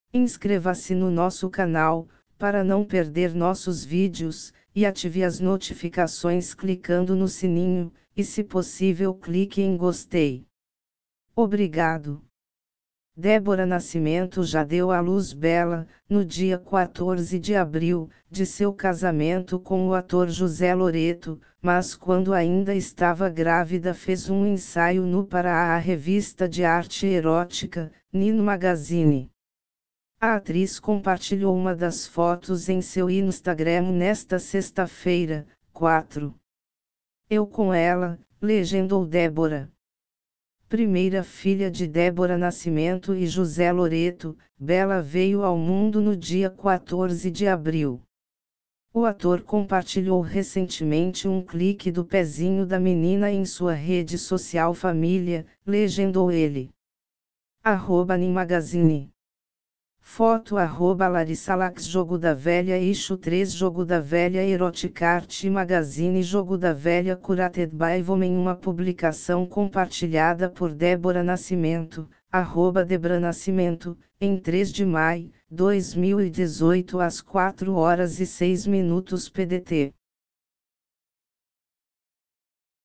Débora Nascimento faz ensaio nu para revista de arte erótica 04.05.2018 09h39
Débora Nascimento  já deu à luz Bella, no dia 14 de abril, de seu casamento com o ator José Loreto, mas quando ainda estava grávida fez um ensaio nu para aa revista de arte erótica, "Nin Magazine". A atriz compartilhou uma das fotos em seu Instagram nesta sexta-feira (4). "Eu com ela", legendou Débora. Primeira filha de Débora Nascimento e José Loreto, Bella veio ao mundo no dia 14 de abril. O ator compartilhou recentemente um clique do pezinho da menina em sua rede social. ""Família", legendou ...